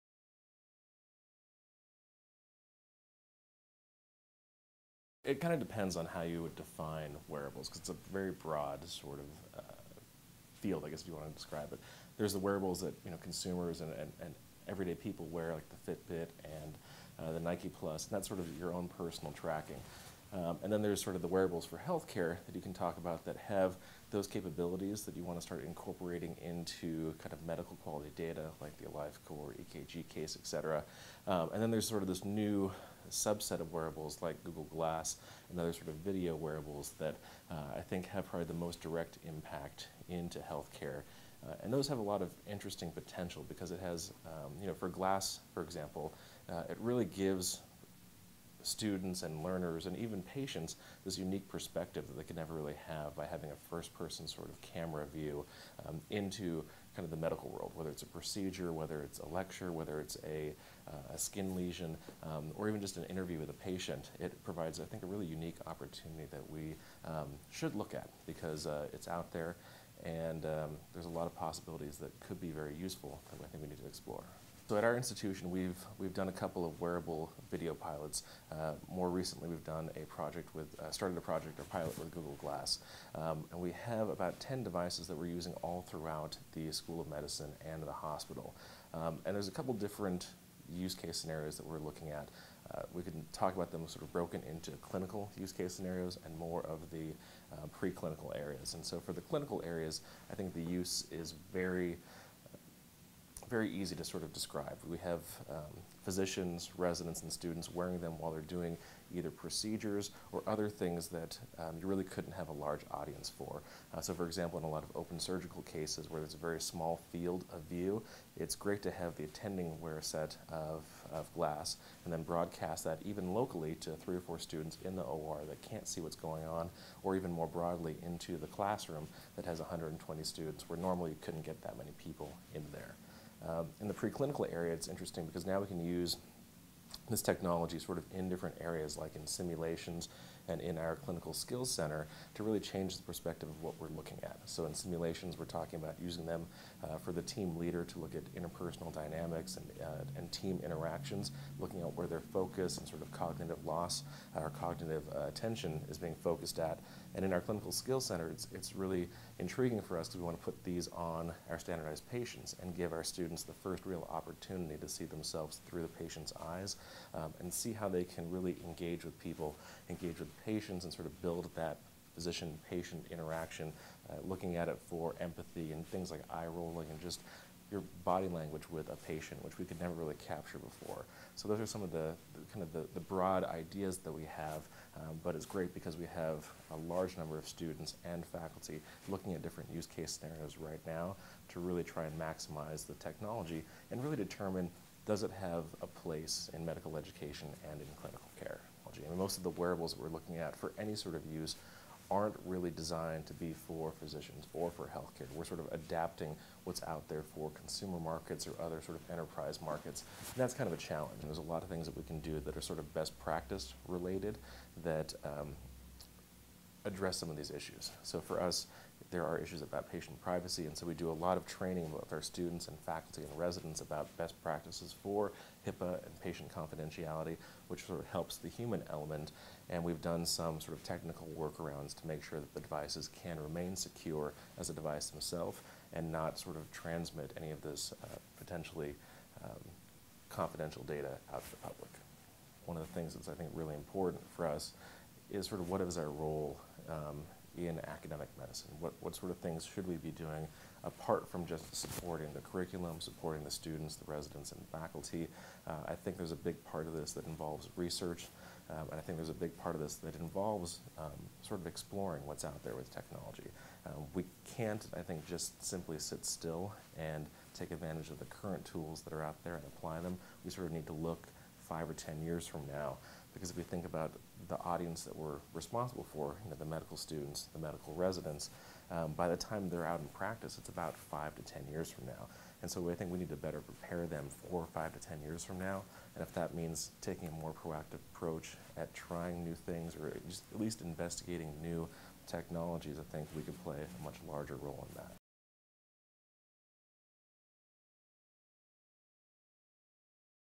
Wearable Technologies in Medical Education and Clinical Care
What role do wearable technologies like Google Glass have in medical education and clinical environments? Listen to Warren Wiechmann, M.D., M.B.A. discuss how UC Irvine is using these tools for improving learning and patient care.